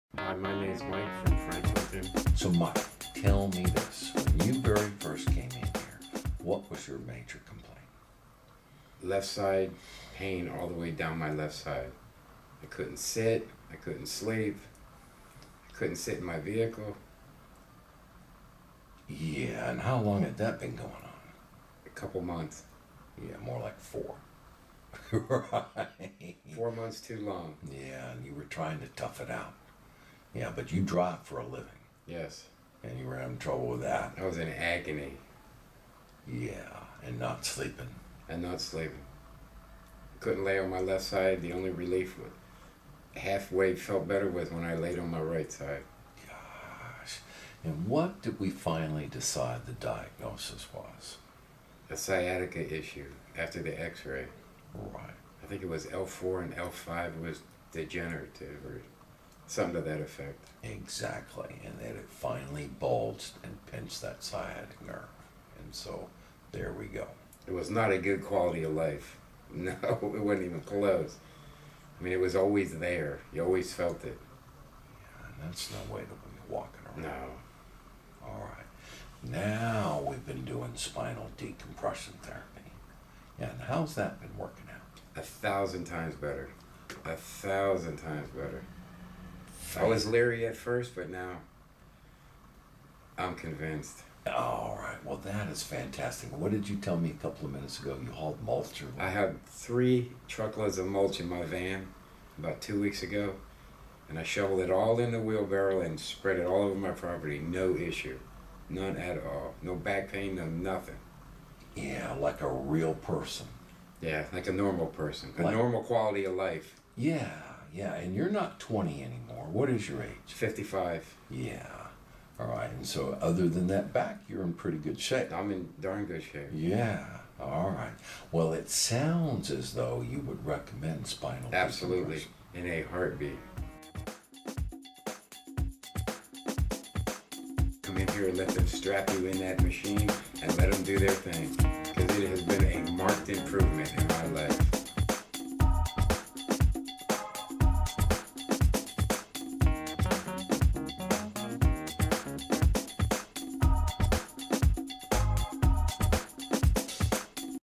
Sciatic Pain Down Leg | Sciatica | Bulging Disc | Back Pain
Triangle Disc Care - Raleigh – Mike had such severe leg pain that he was in agony. Now he is 1000 times better and can even shovel mulch. He would recommend Spinal Decompression Therapy in a heartbeat.